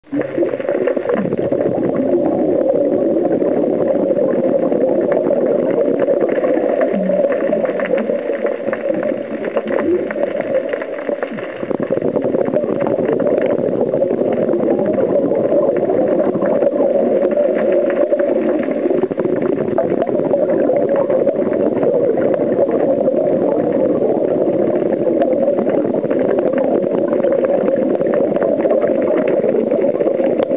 Porcupine Puffer gets curious about the camera
Porcupine puffers or Balloonfish--They are everywhere and make great subjects for the camera. Learn more about about diving and snorkeling in Manzanillo with Scuba Shack, or snorkelmanzanillo.com